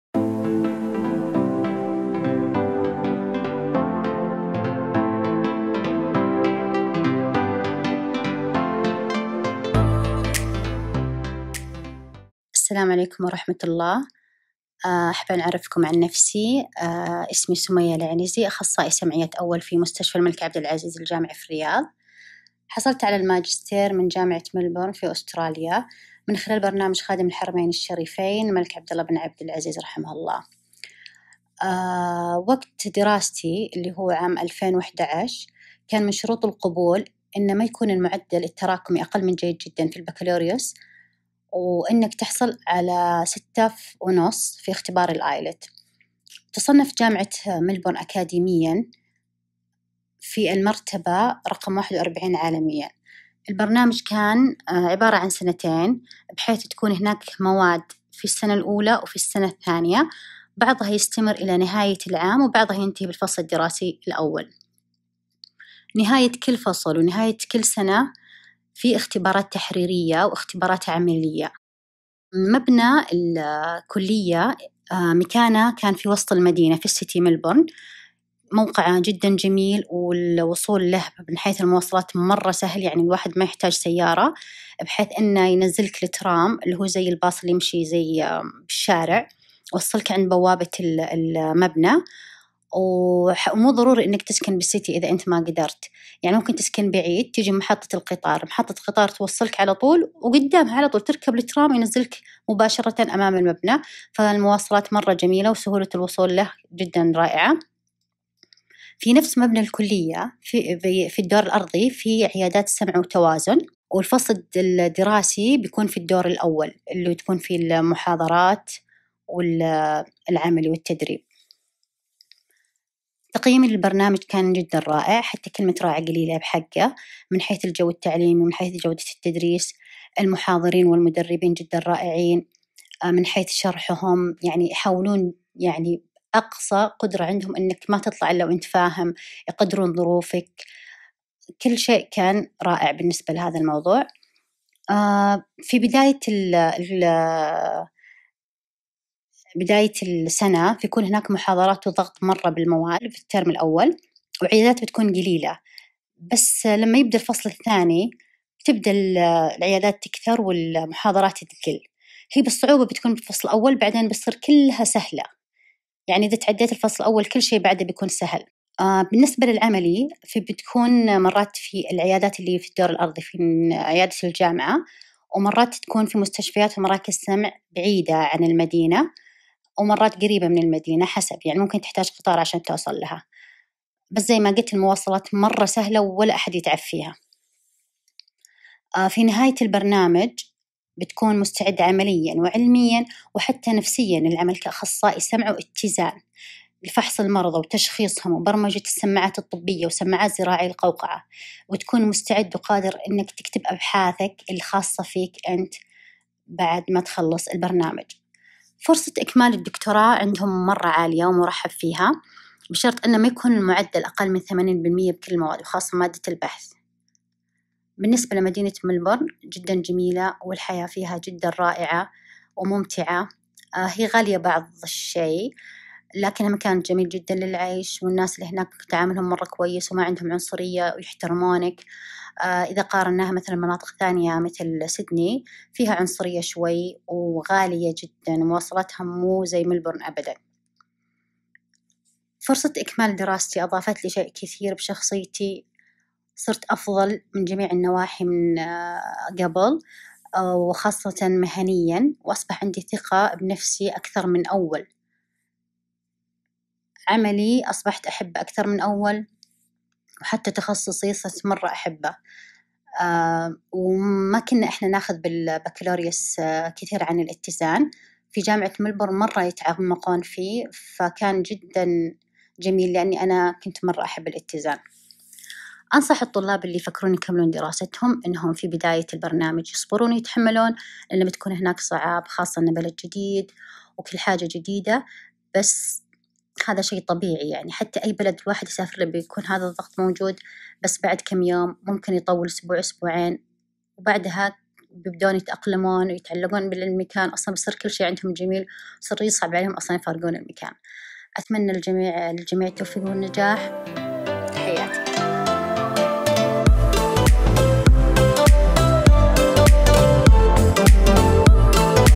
سلسلة تعريفية | ماجستير السمعيات والتوازن - دولة أستراليا مع أ. سمية العنزي
سلسلة من المقاطع المصورة تهدف إلى:
تكوين فكرة عن المسارات المهنية المتاحة لطلاب الثانوية الراغبين في دخول مجال السمعيات.
زيادة وعي المتسجدين في المجال والمجتمع حول الكفاءات السعودية العالية في مجال السمعيات والتوازن.
تسهيل عملية البحث عن برامج الدراسات العليا للمهتمين من المتخصصين السعوديين والعرب.
إثراء المحتوى العربي فيما يخص برامج الدراسات العليا في مجال السمعيات كونها السلسلة العربية الأولى من نوعها.